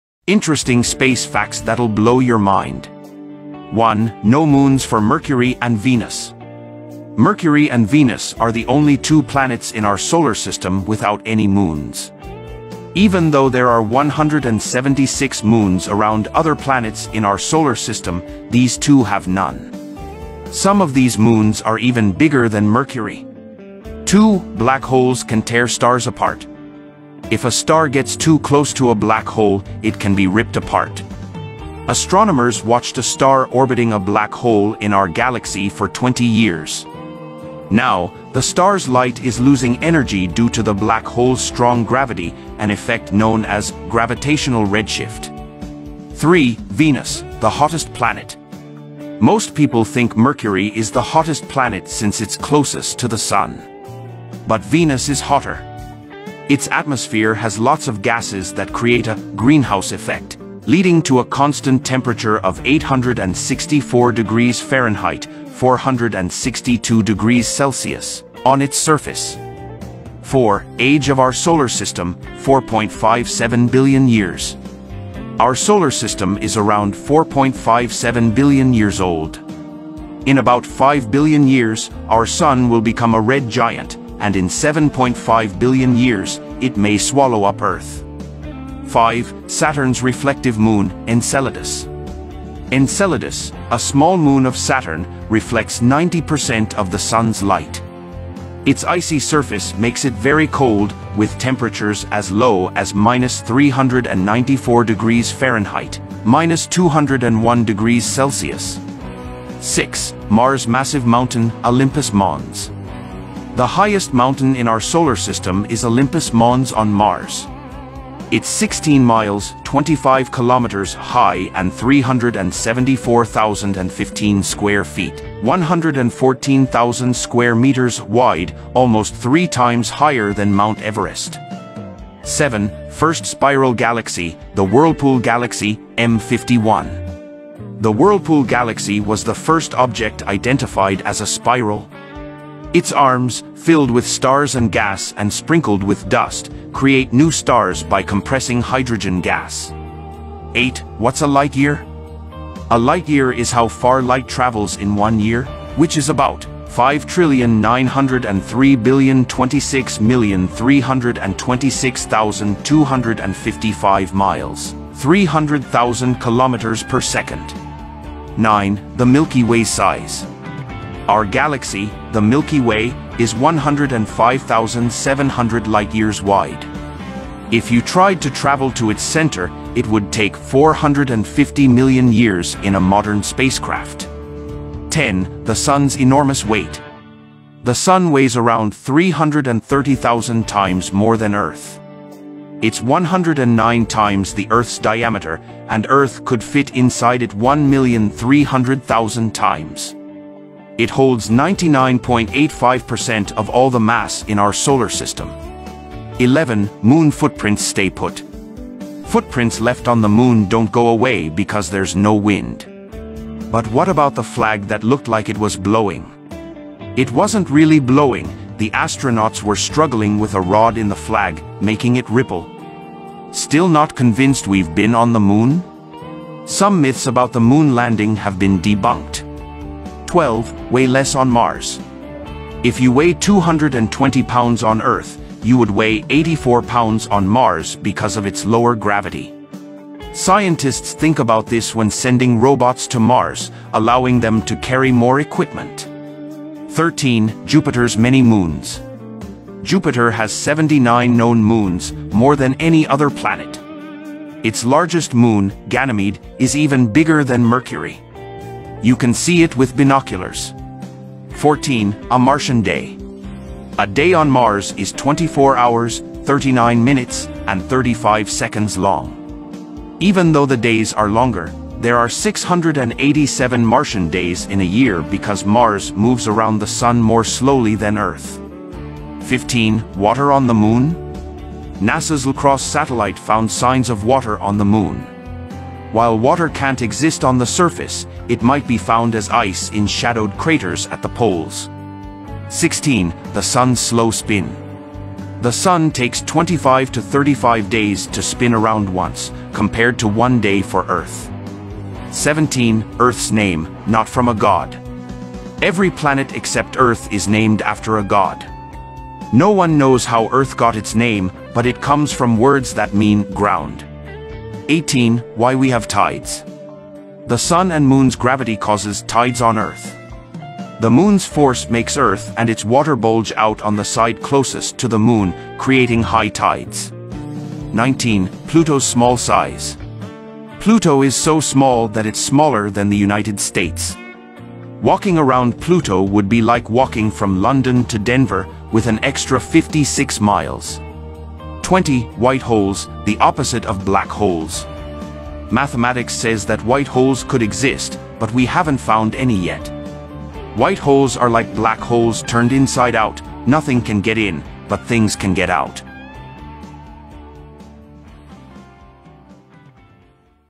Interesting Space Facts That'll Blow Your Mind
Title: "🚀 Explore the Cosmos: Mind-Blowing Space Facts Revealed! 🌌"

Description:
"🌠 Brace yourself for an interstellar journey as we delve into the fascinating universe with mind-blowing space facts! From mind-bending dimensions to cosmic wonders, this video is your ticket to a cosmic adventure. 🚀✨

🪐 Did you know that a teaspoon of a neutron star weighs as much as Mount Everest? Join us as we unravel the mysteries of these incredible celestial bodies.

🔭 Explore the mind-bending concept of black holes, where time itself can warp and twist. Learn about their voracious appetite for everything, even light!

🌌 Venture into the mesmerizing world of exoplanets, planets beyond our solar system, and discover the potential for alien life!

🌈 Unearth the beauty of nebulae, colossal clouds of gas and dust where stars are born and die in breathtaking cosmic displays.

🌑 Ever wondered what happens during a supernova? Dive into the explosive spectacle of dying stars, releasing energy equivalent to billions of suns.

🛰️ Learn about the incredible speed of light and how it helps us peer into the distant corners of the universe, revealing its age and vastness.

🌕 Marvel at the Moon's impact on Earth, influencing tides and serving as a silent witness to our planet's history.

🪐 Join us on this cosmic rollercoaster as we unravel these mind-bending space facts that will leave you in awe of the vast, mysterious universe. 🚀🌌✨ SubscribeNow CosmicAdventure SpaceExploration DiscoverTheUniverse"